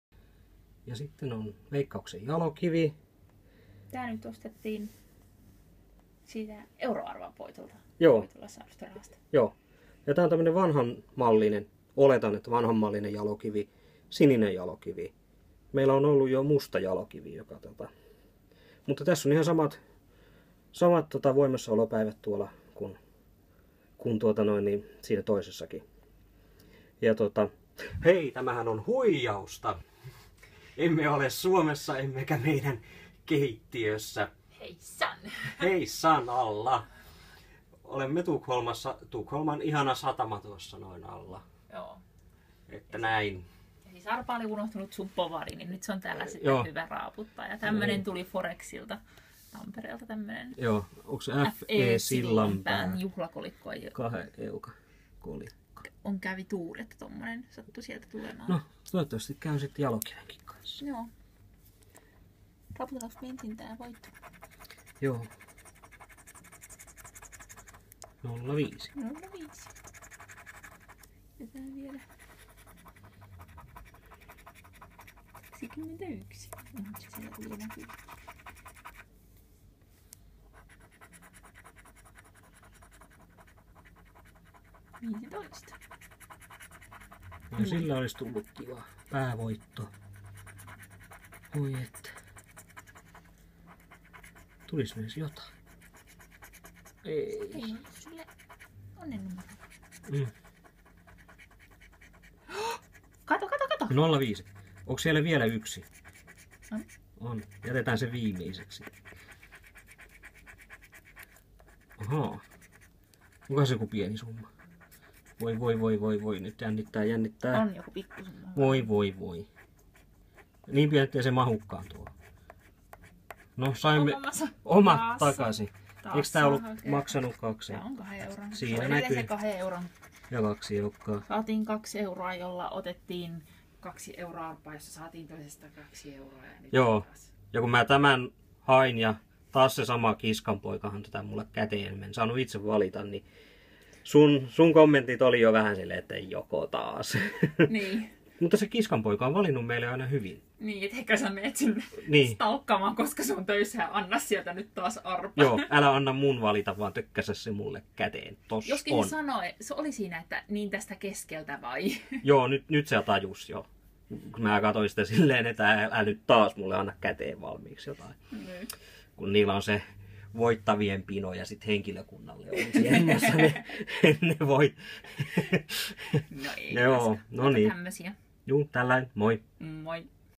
Veikkauksen Jalokiviarpa
Perjantaivideona raaputetaan Veikkauksen Jalokivi keittiömme pöydän ääressä ;)